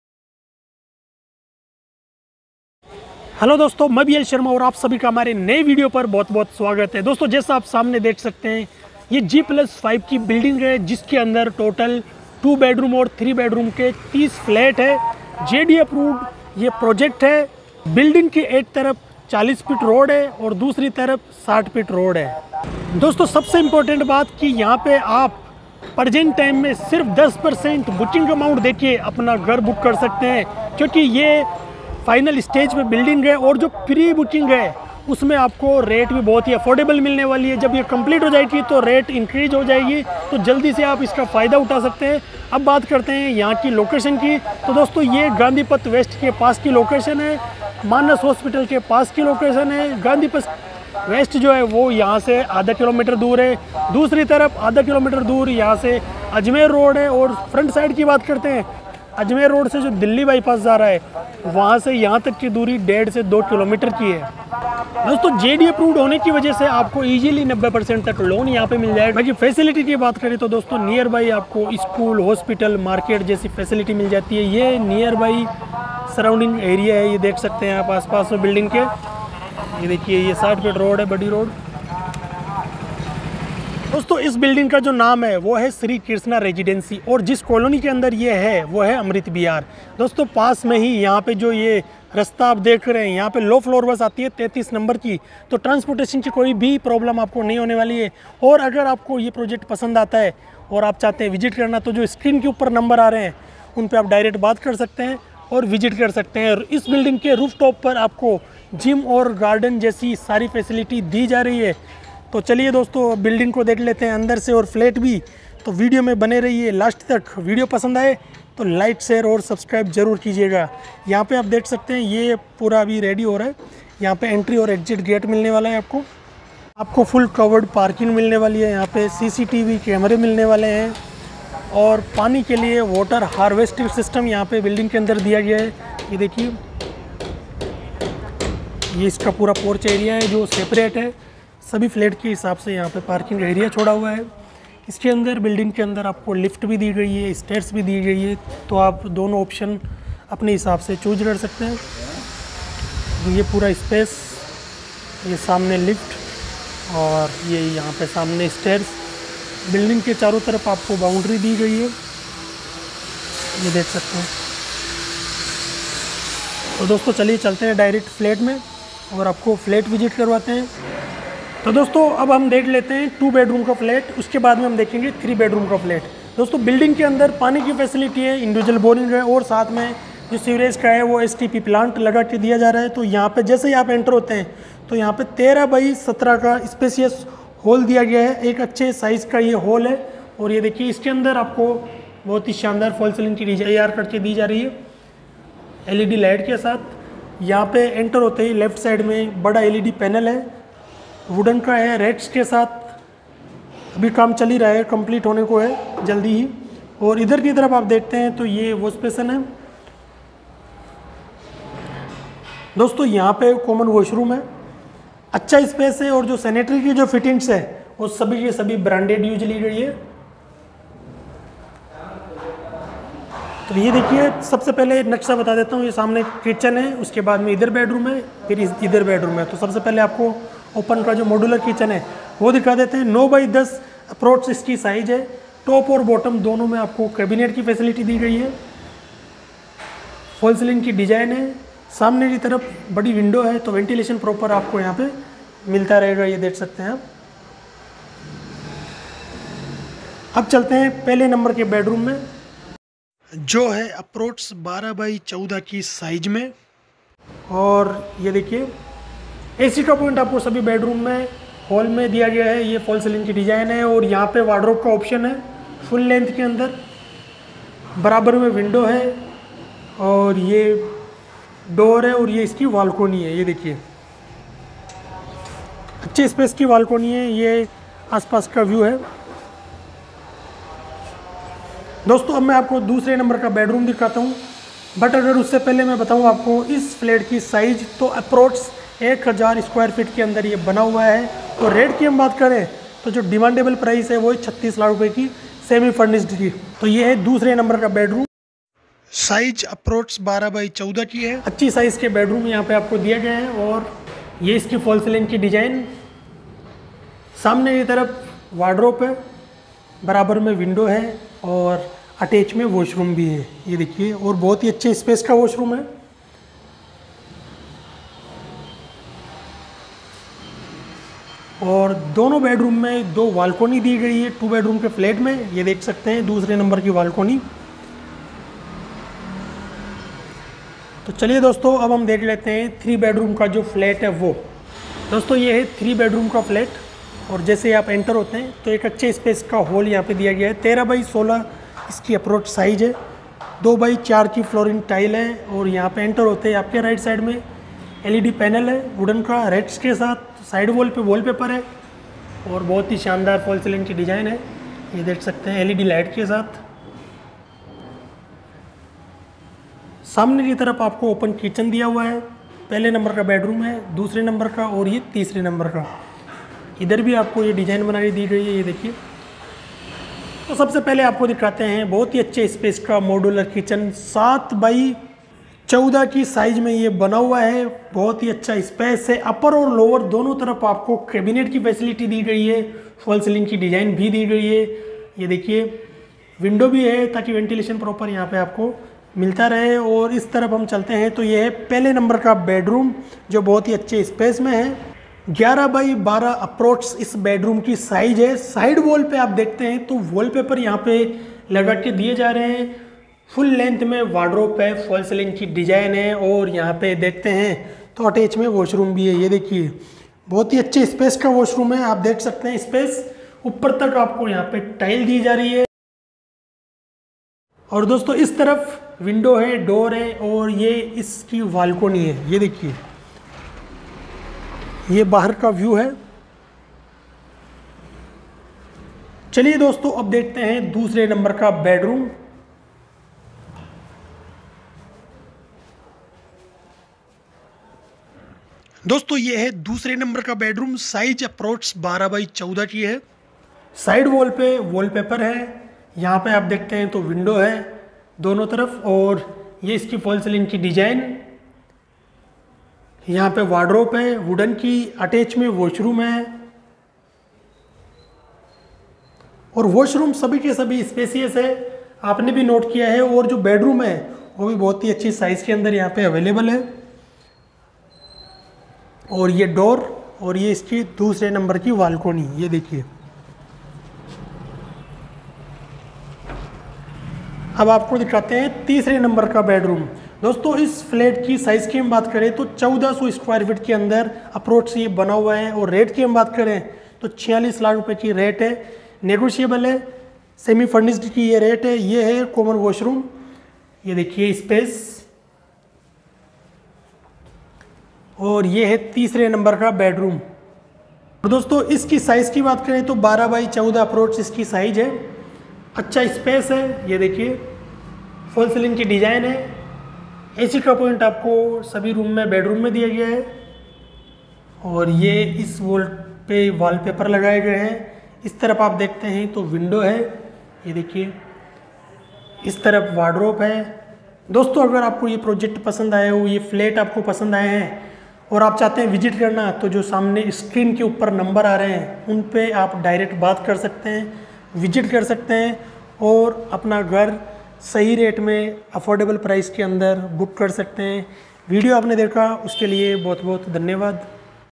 सिर्फ 10% PAYMENT पर LUXURY घर ख़रीदे | PROPERTY IN JAIPUR FOR SALE
Property Details
2 & 3 BHK Flats
location Near Manas Hospital Gandhipath west Vaishali nagar jaipur
2 BHK Size 1000 Sq.Feets Price 36 Lakh
3 BHK Size 1400 Sq.Feets Price 46 Lakh
Jda Approved
All Facilities